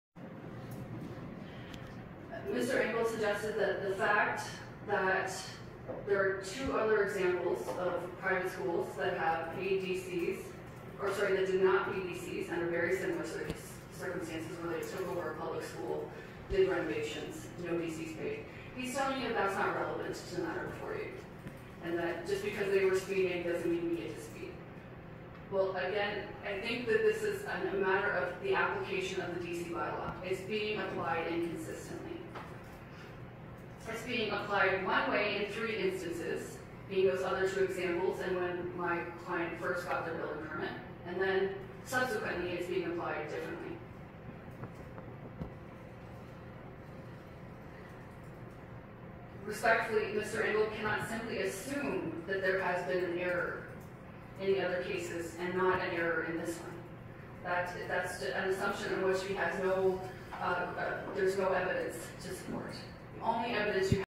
Raw Video: Hamilton Councillor Mike Spadafora in motor vehicle during meeting (Video 4)
Recording begins at 13:28:34 on August 21, 2024, during a meeting of the Audit, Finance, and Administration Committee.
Jennifer Meader, lawyer for a tax appellant, is making closing arguments while Ward 14 Councillor Mike Spadafora's attention is diverted multiple times.
Spadafora said he was not operating the motor vehicle.
His camera may be mirroring his appearance.